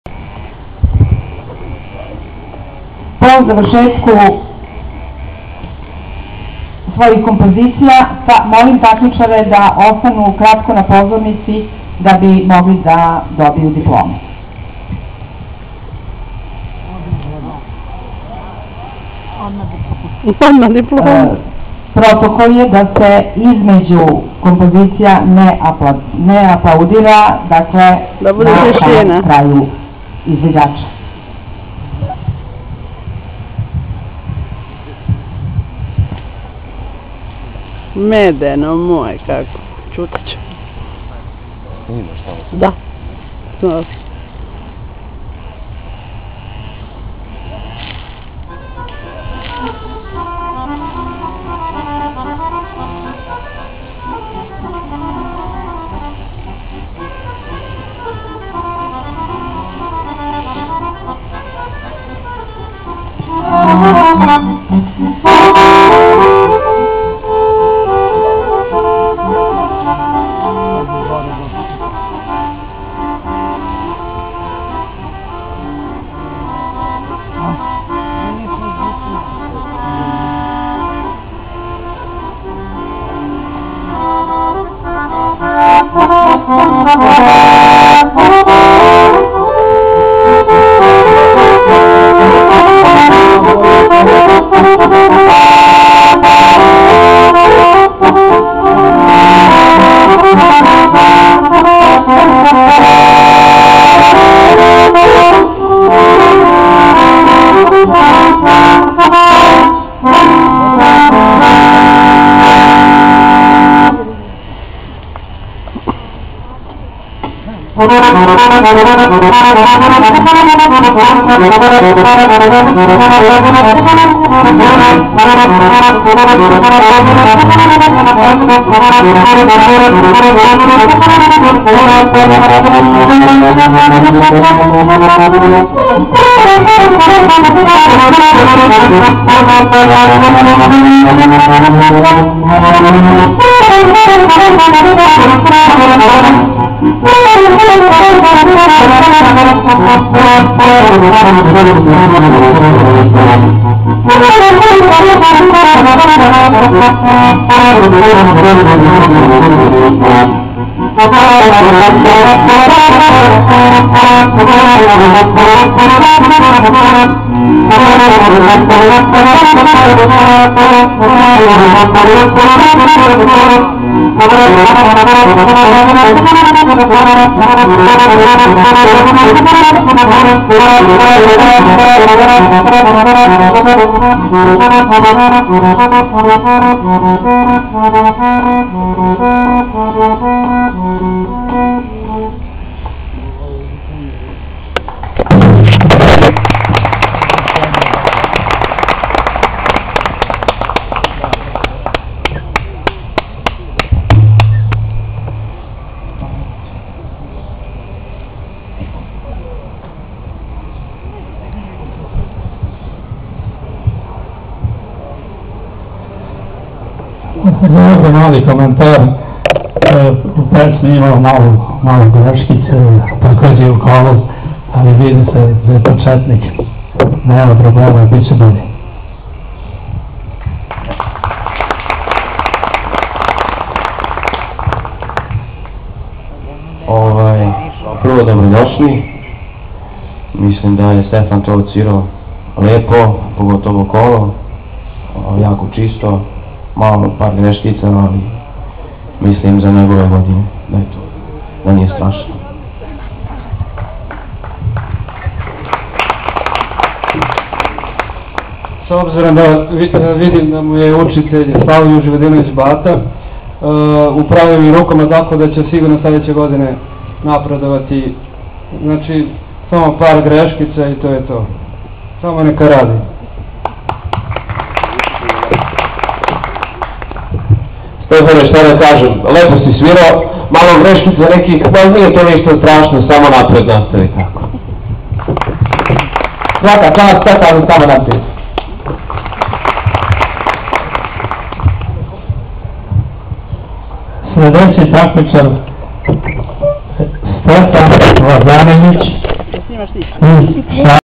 PRVA HARMONIKA SRBIJE JAGODINA 2013 STEFAN PAVLOVIC
prva harmonika srbije jagodina
kategorija petlica prvo  mesto
Stefan Pavlovic 8 god   Popadic    Mionica
ucenik Darka Vujicic